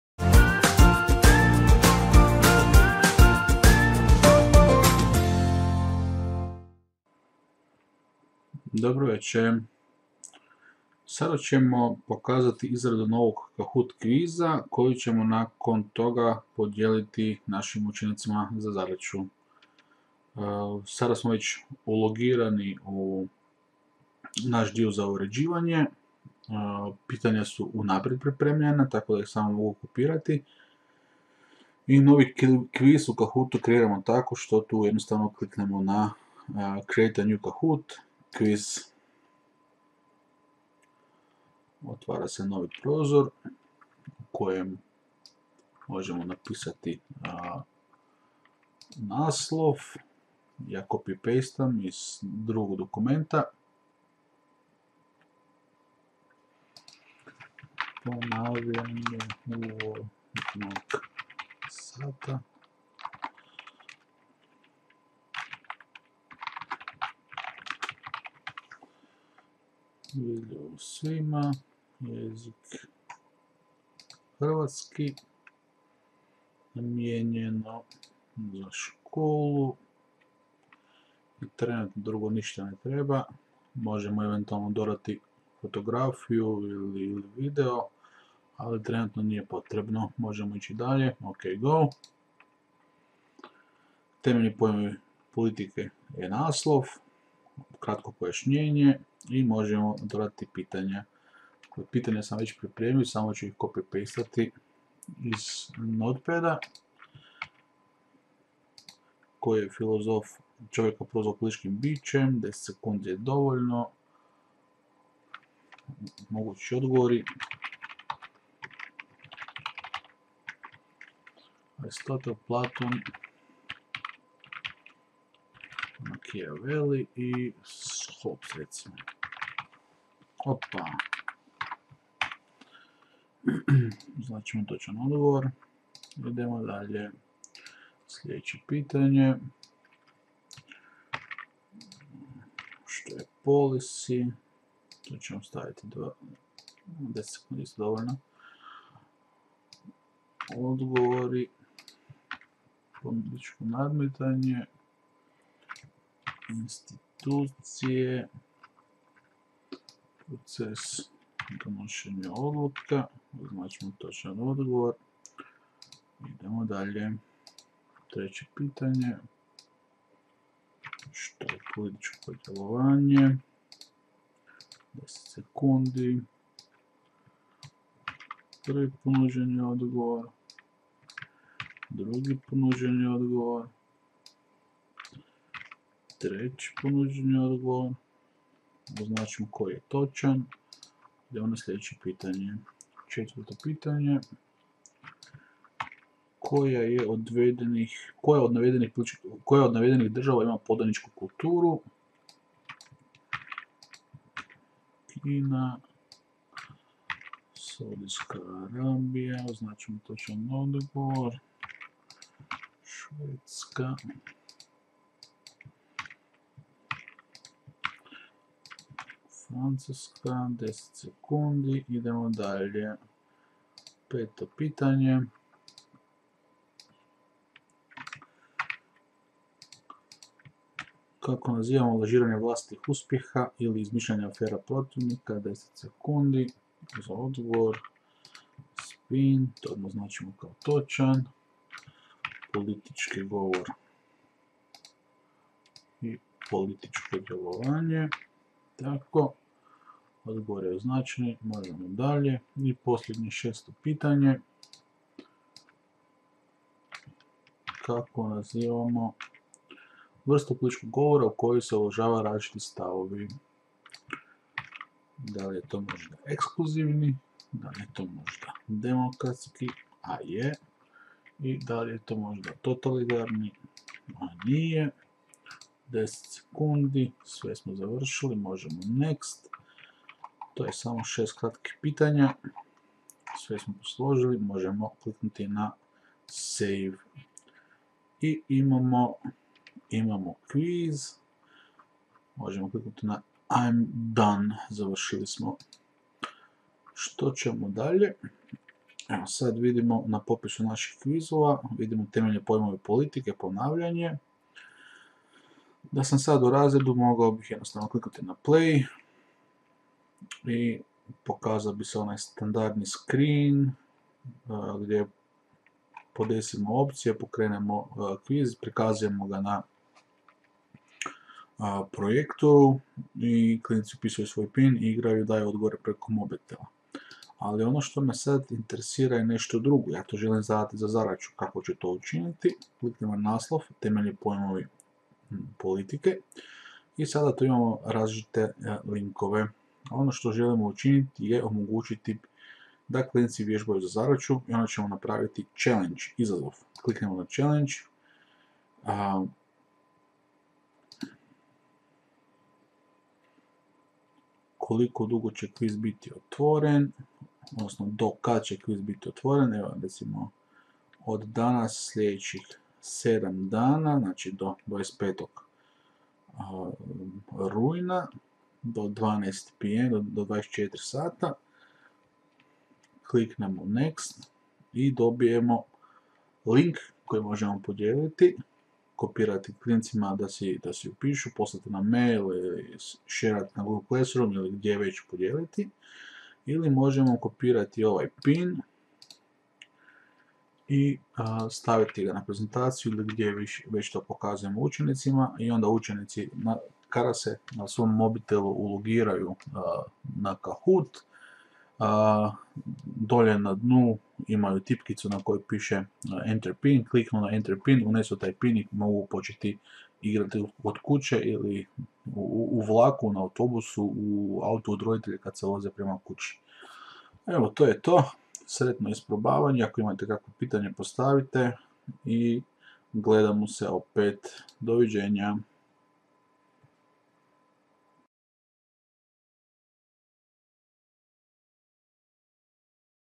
Izrada novog Kahoot kviza i podjela zadaće
Kako se izrađuje Kahoot kviz i kako ga učenicima podijeliti za zadaću?

Ukoliko imate pitanja o Kahootu postavite ih u komentare.

Svaki tjedan na ovom kanalu možete očekivati neku novost iz svijeta poučavanja uz pomoć IKT-a. Gledamo se!